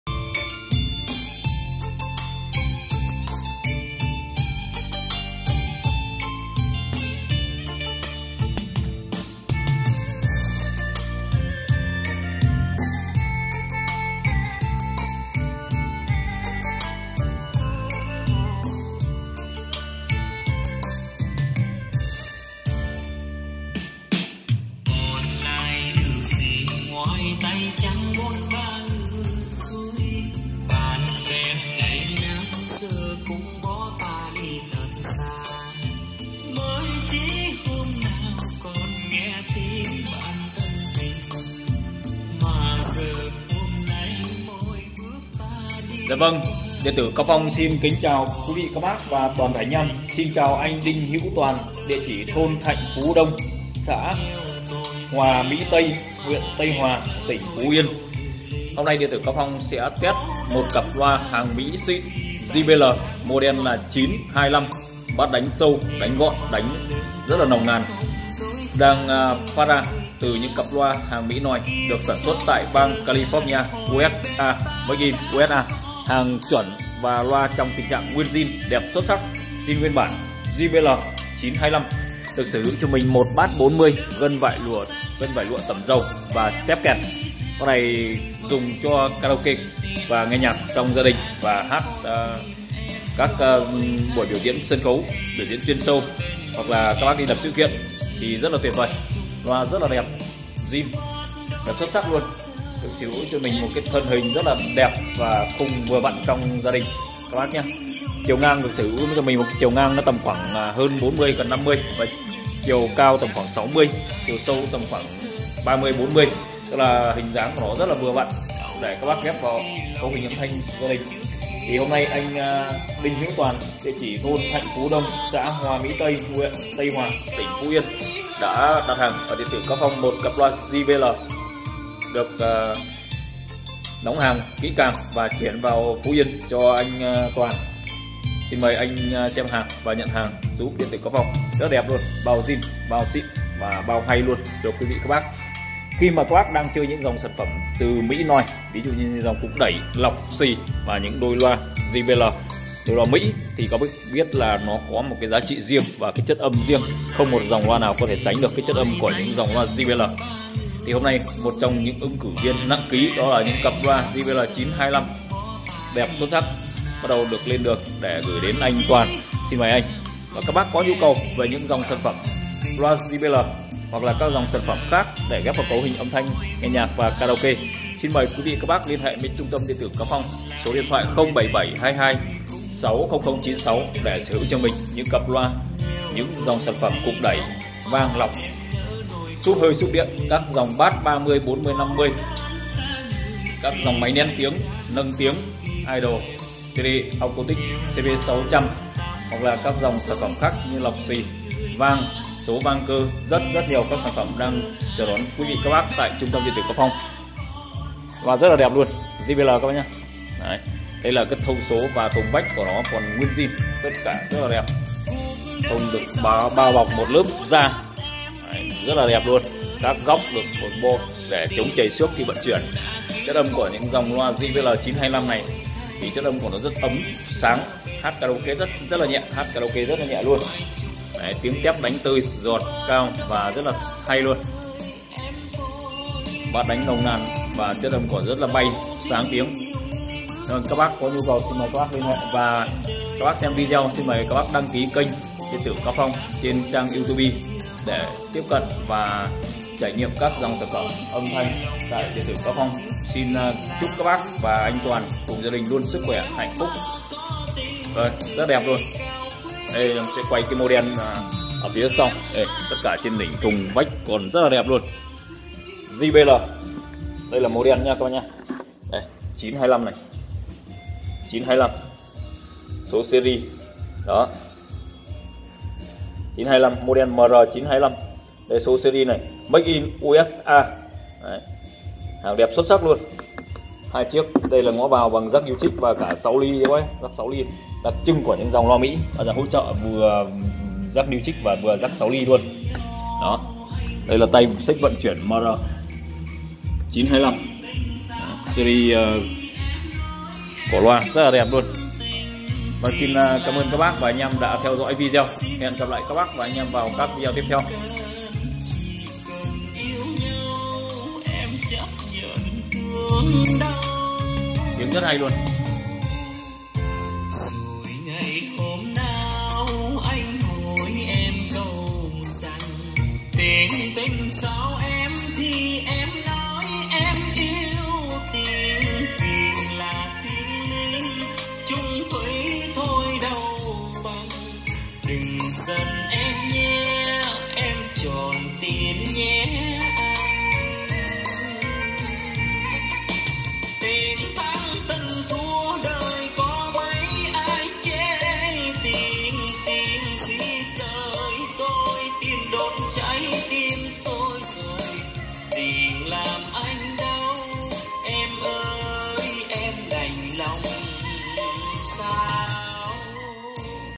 loa mỹ nòi JBL-925.đẹp xuất sắc,zin keng,chất âm đỉnh cao,gửi a toàn-phú yên.liên hệ:0772260096
trung tâm điện tử cao phong chuyên cung cấp các thiết bị âm thanh nghe nhạc và hát karaoke gia đình và karaoke chuyên nghiệp,ship hàng toàn quốc.điện thoại ☎